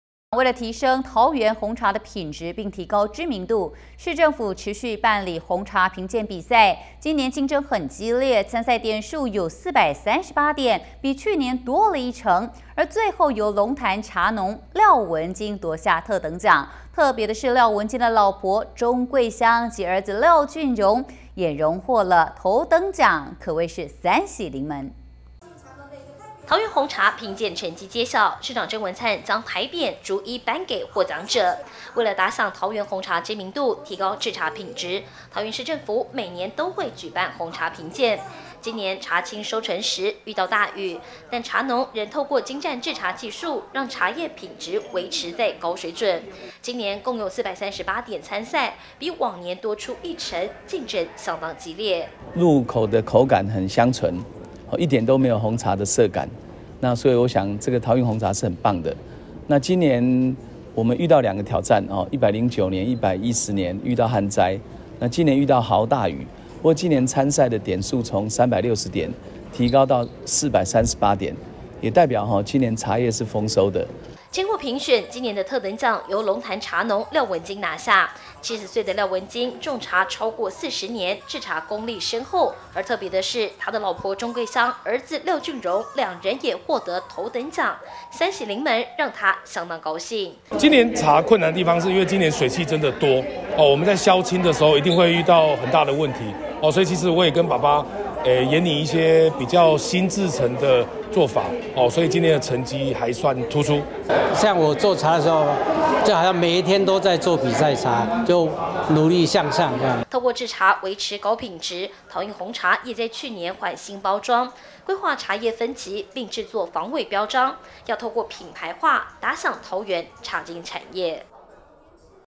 桃園市紅茶評鑑 廖文金奪特等獎！│T-NEWS聯播網
為了提升桃園紅茶品質並提高知名度，市政府持續舉辦紅茶評鑑比賽；今年競爭激烈，參賽點數有438點，比去年多了1成；最後，由龍潭茶農廖文金奪下特等獎。特別的是，廖文金的老婆鍾桂香及兒子廖俊榮，也獲得頭等獎，可謂三喜臨門！